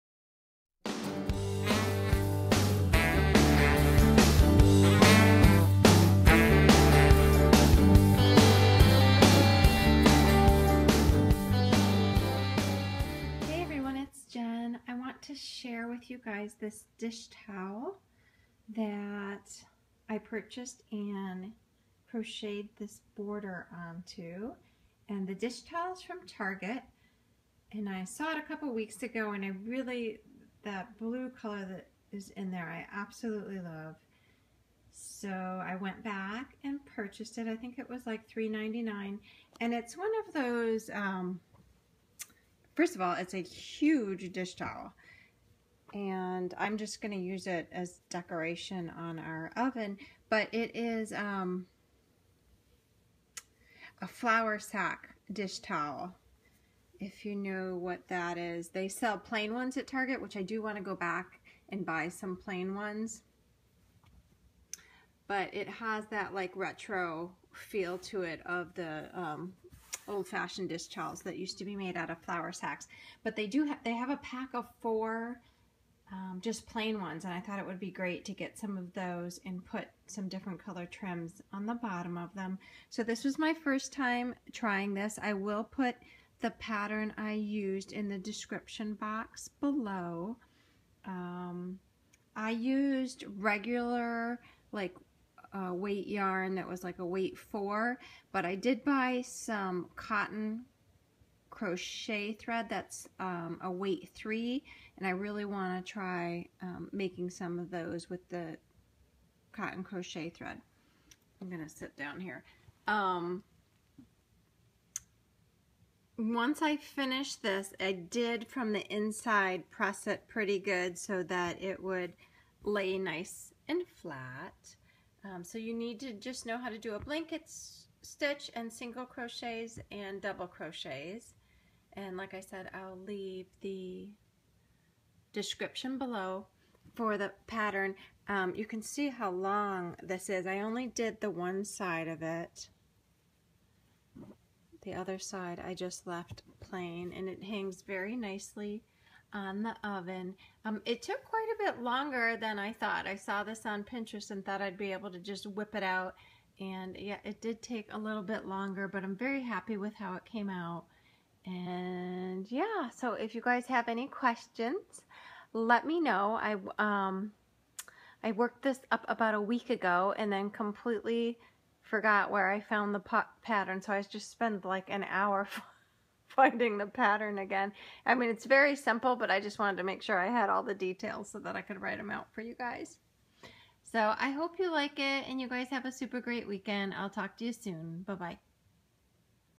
Crochet Edged Dish Towel
Added this crochet edging to a retro flour sack towel I purchased at Target.

Crochet Trim Pattern:

I used yarn weight 4 with an E/4-3.5mm hook.

Blanket stitch every 1/2 inch using yarn needle.

Row 1: SC 3 times in-between each blanket stitch